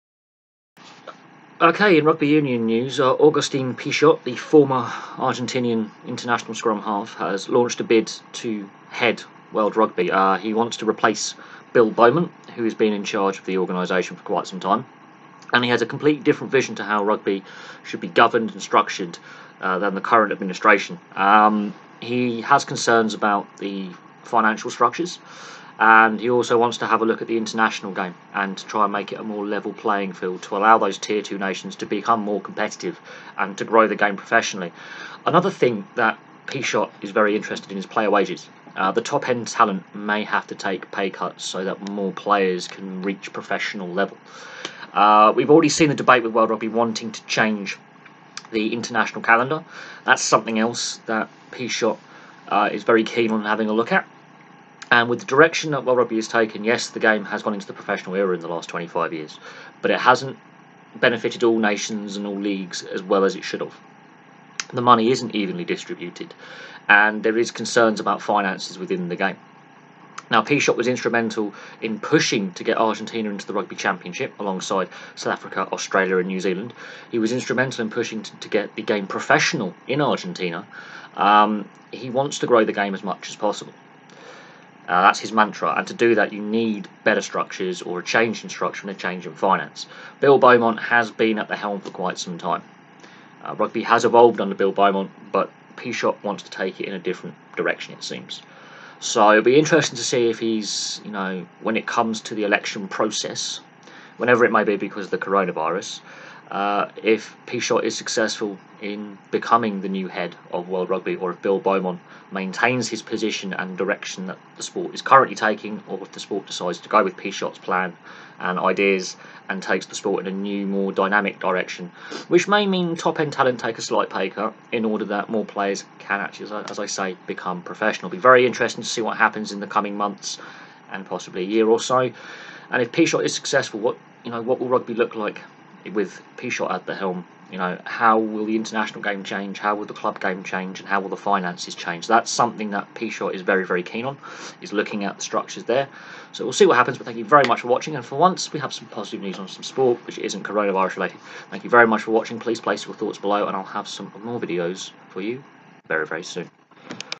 Augustin Pichot Launches Bid To Become Head Of World Rugby
He wants to replace current head Bill Beaumont who has been in the post for many years. His platform is based on restructuring the finances  and the structures of the sport.